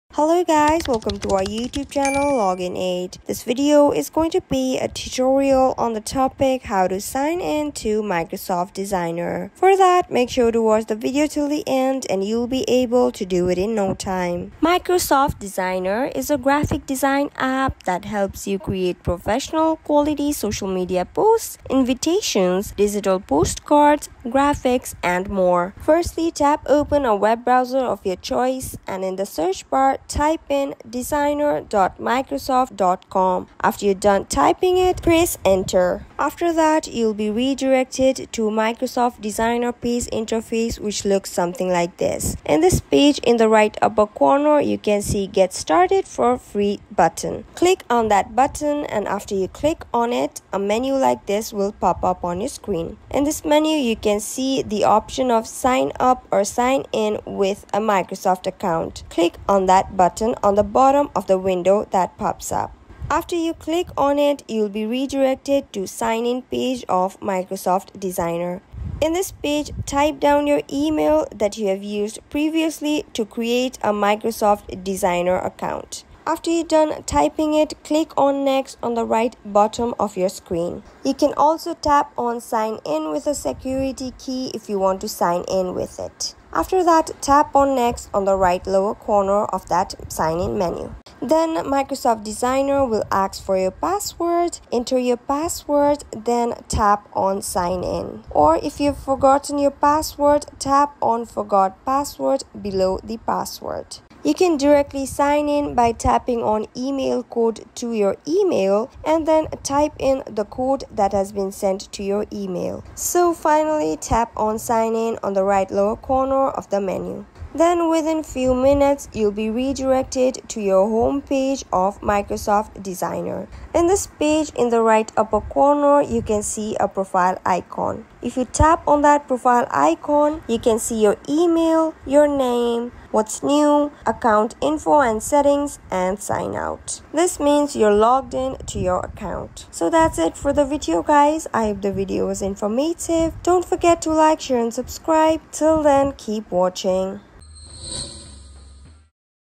How To Login Microsoft Designer Account 2023 | Microsoft Designer Sign In Guide
How To Login Microsoft Designer Account 2023 | Microsoft Designer Sign In Guide | designer.microsoft.com

A video guide for the proces to login Mircosoft Designer account.